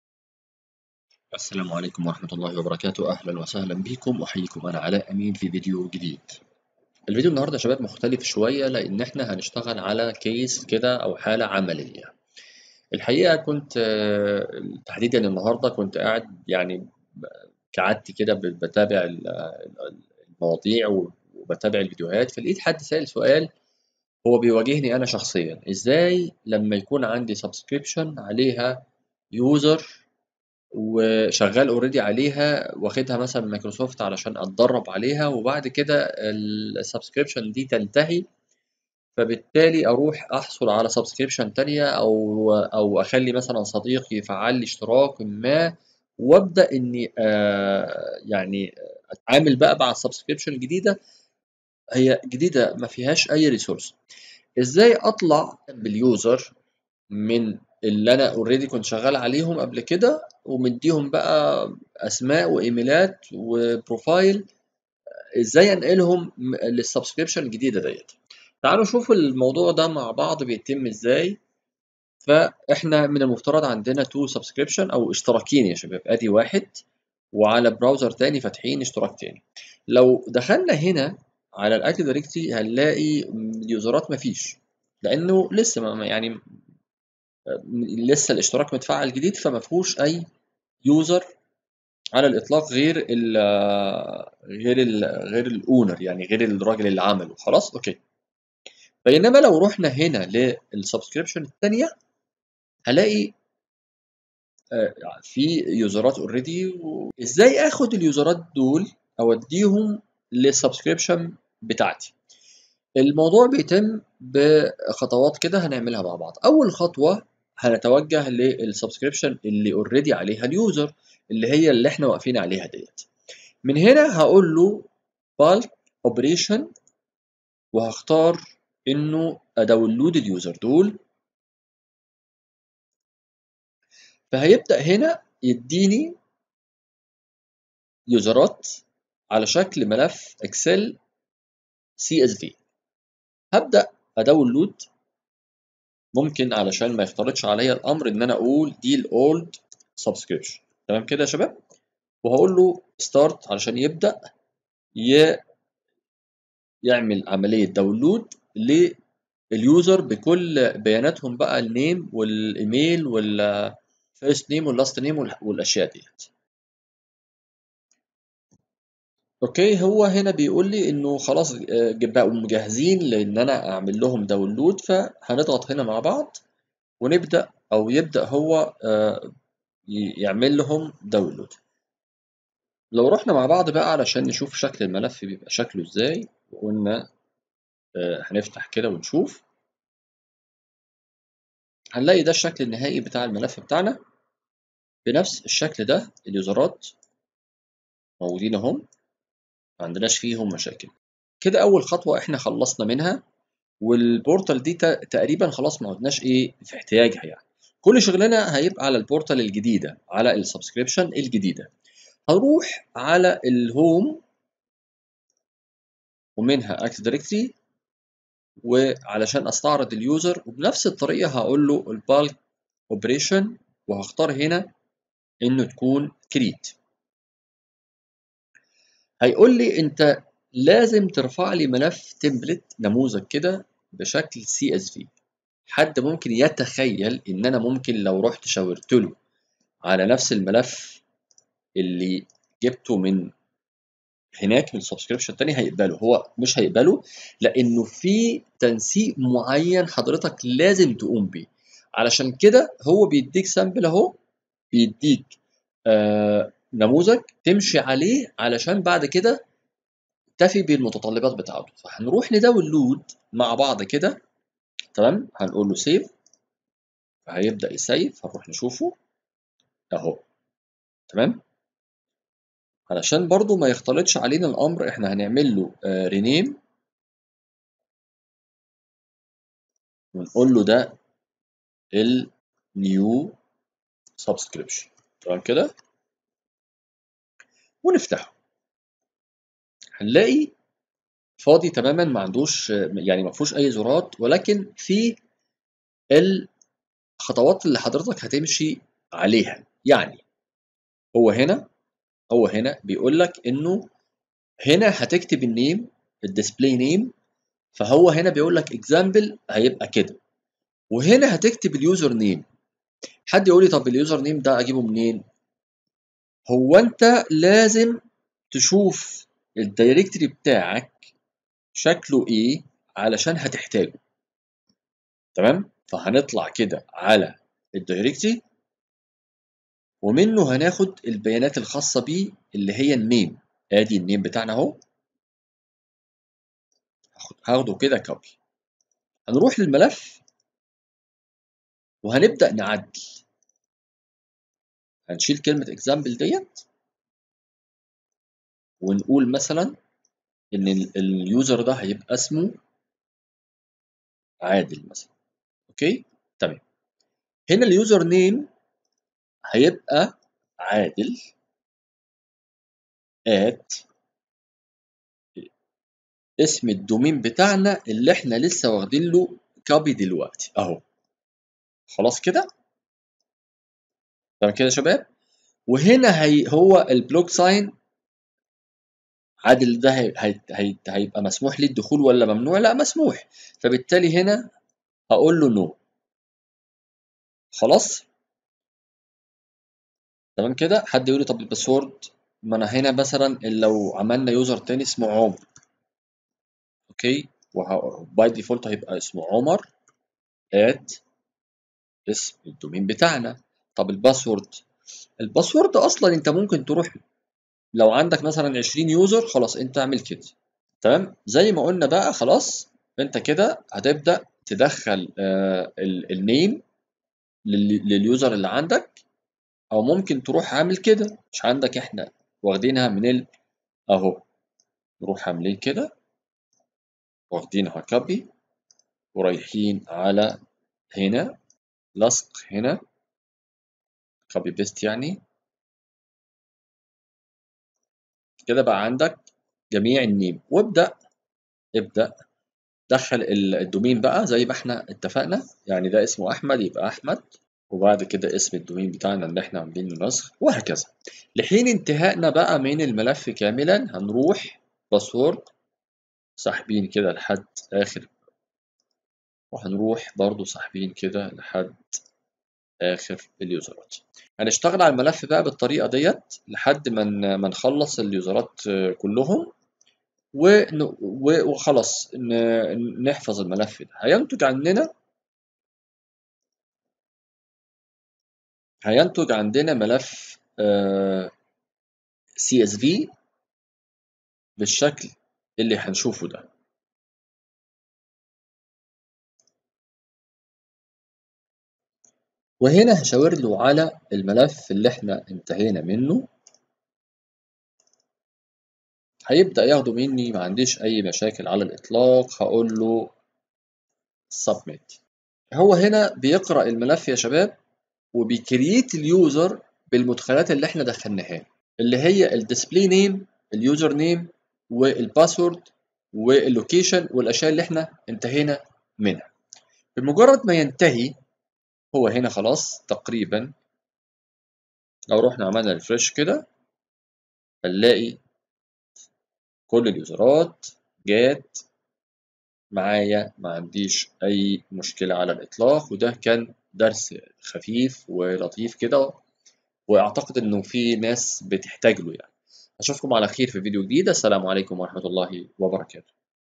Azure Bulk Operations
في هذا الفيديو نستعرض معا كيفية القيام بالعمليات المجمعة على المستخدمين
فبدلا من انشاء كل مستخدم على حده يمكن القيام بانشاء عدد كبير منهم دفعة واحدة